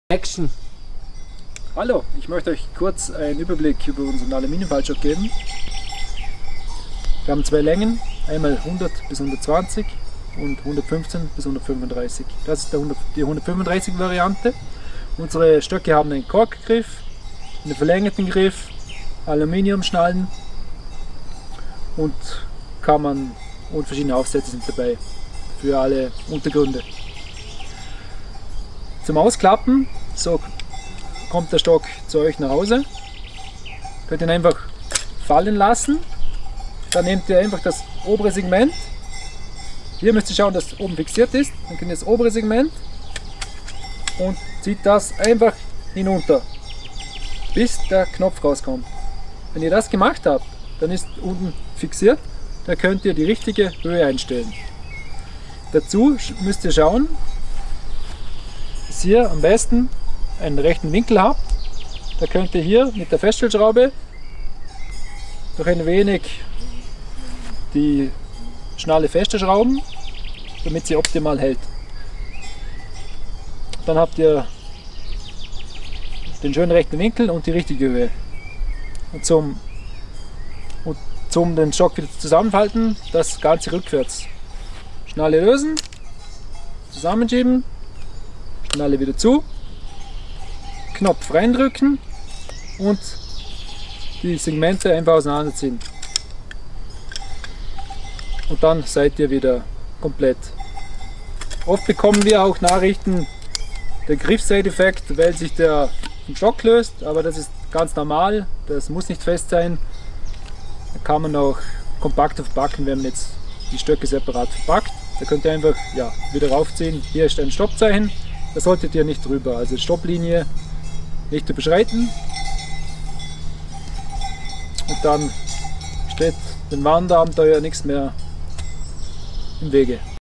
ALPIN LOACKER - Allmountain Aluminium Faltstöcke / Anleitung
Du suchst Aluminium Faltstöcke leicht, stabil, mit kleinem Packmaß und ergonomischen Griffen aus Kork? Dann bist du bei den Alpin Loacker Allmountain Series Wanderstöcken völlig richtig. Unsere Aluminium Wanderstöcke bieten das beste Preis-Leistung-Verhältnis am Markt. Zum Wandern, Bergsteigen oder Skitouren perfekt geeignet. Der Faltstock aus Aluminium.

Produktinformationen:

Verstellbreich Standard (ab Körpergröße 165cm): 115cm bis 135cm
Verstellbreich kurz (bis Körpergröße 165cm): 100cm bis 120cm
Länge zusammen: 35cm
Eigengewicht: 582g pro Stock 291g
Faltbar
Material: ultra leichtes Flugzeug Aluminium
Zubehör: 2 verschiede Teller + Tasche + Gummispitzen
Einsatz: Wandern, Bergsteigen, Skitouren
2 Jahre Garantie